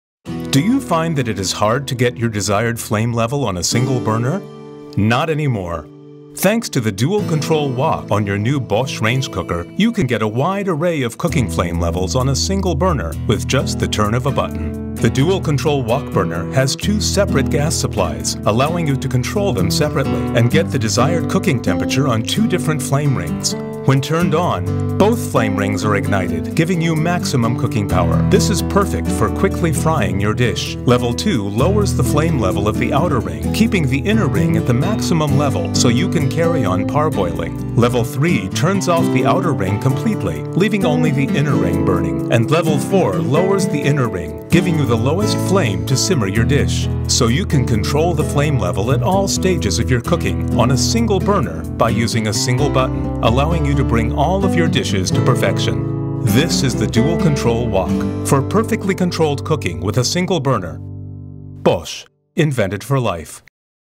Bosch Cooker DualControl Wok Explained
The Dual Control Wok allows you to have a precise cooking experience via two adjustable flame rings controlled by one knob, for instance, this function is perfect for cooking rice where you need all levels to be working simultaneously, leaving you take full control of your day-to-day cooking.

Aimez-vous votre Bosch ? Découvrez-en plus en ligne !